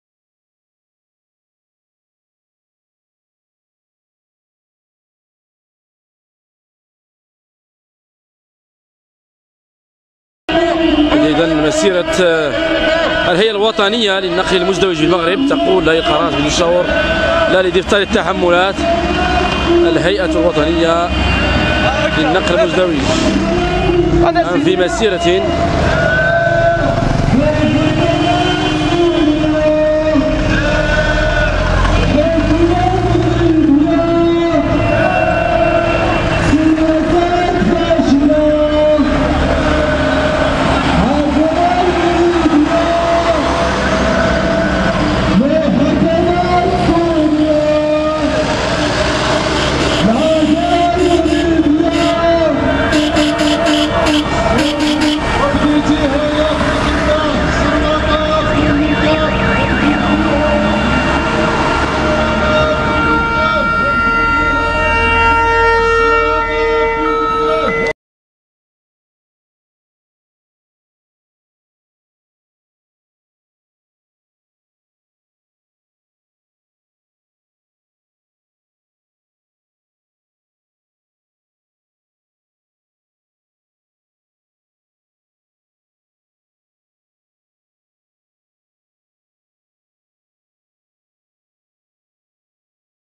مسيرة الهيئة الوطنية للنقل المزدوج - تنغير في فاتح ماي
بعدسة  : حسن  أعبدي .
فاتح  ماي 2014  بمركز  مدينة  تنغير .